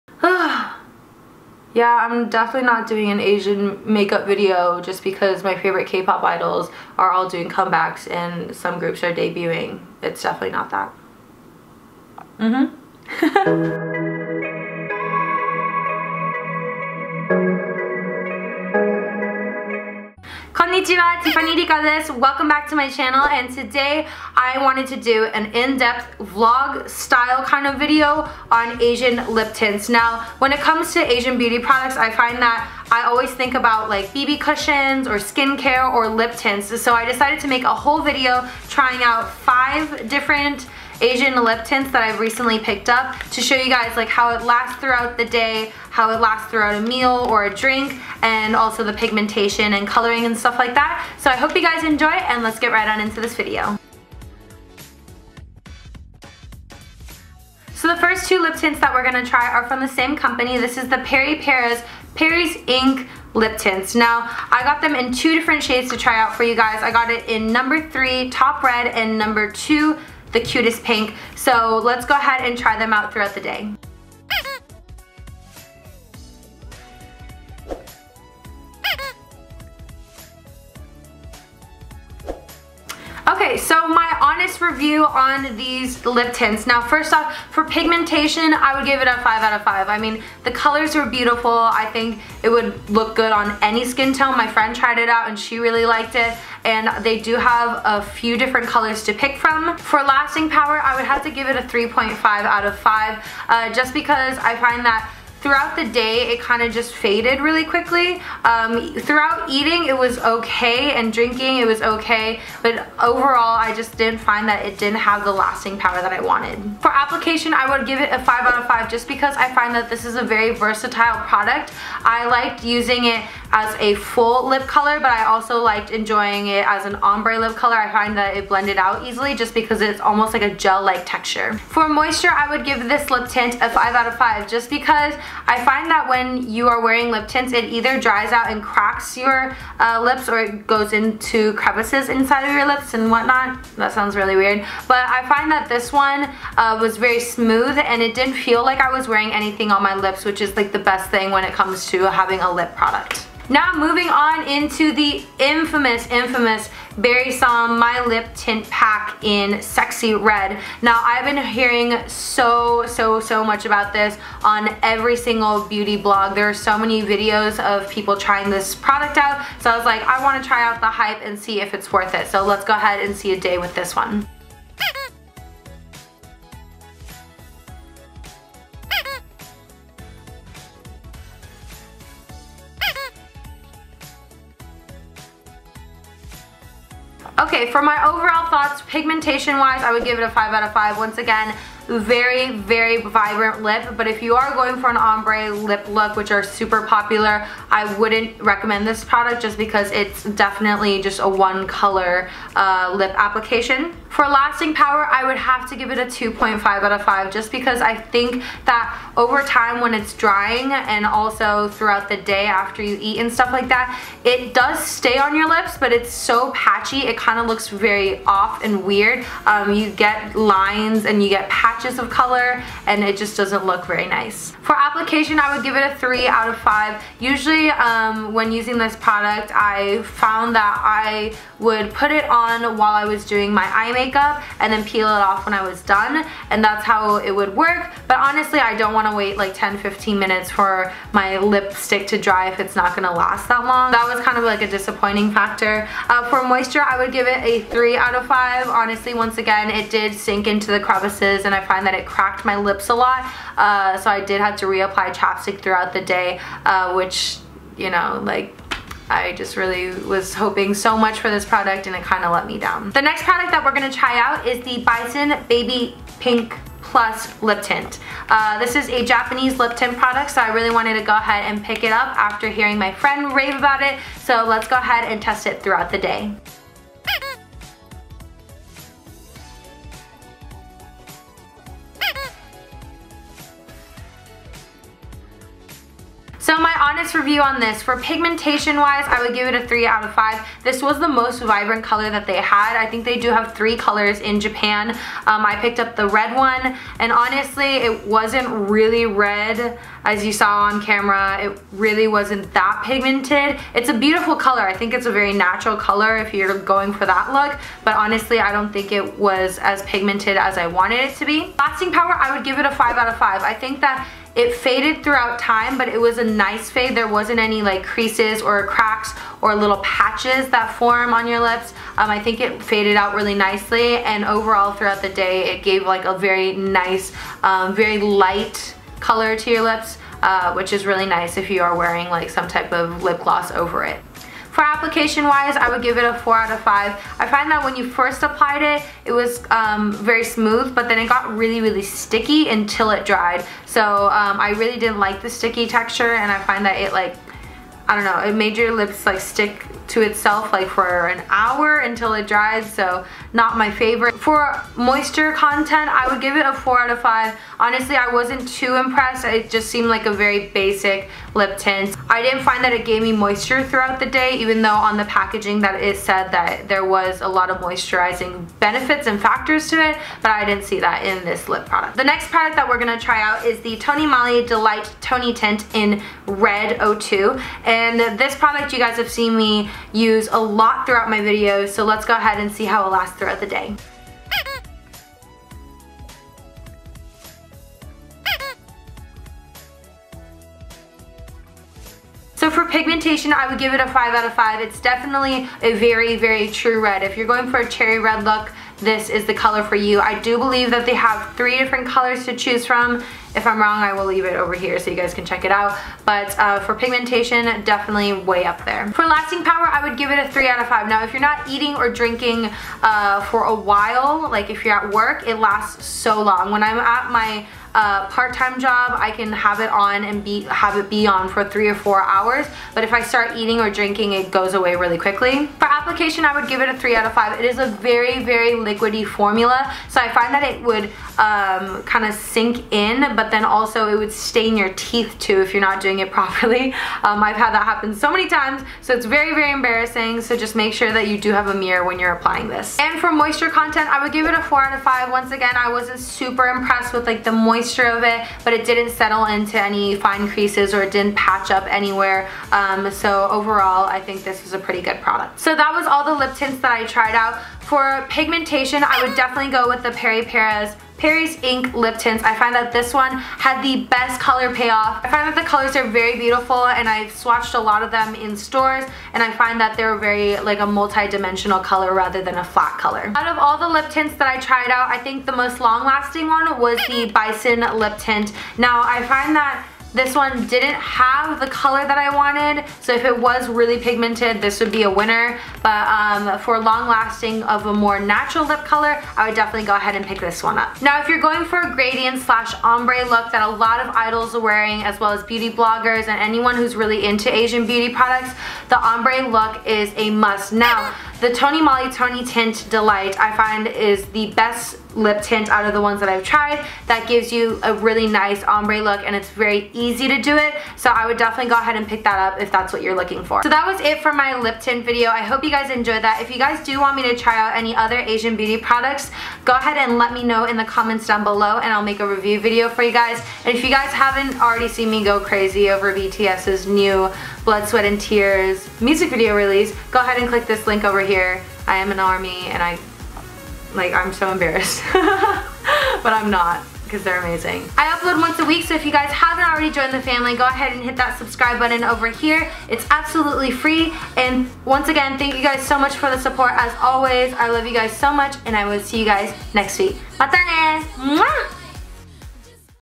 ASIAN LIP TINTS ♡ Haul, Review and Swatches
こんにちは！ティファニー 梨香です！Hi Everyone! It’s Tiffany Rika ♡
When it comes to Korean and Asian beauty must haves, I think lip tints and cushions come to everyone’s minds. So I wanted to take you through a week of testing out some of the most popular and talked about lip tints from idols, beauty bloggers and lovers. Let’s see how these products test out pigmentation, lasting power, moisture, application and even the packaging.

PERIPERA Peri’s Ink [02]

PERIPERA Peri’s Ink [03]

Berrisom Oops Lip Tint [Red]

Tony Moly Delight Tony Tint [Red]

Bison Baby Pink Plus Lip Tint [02 Red]

またねー
xoxo
Tiffany 梨香 Russell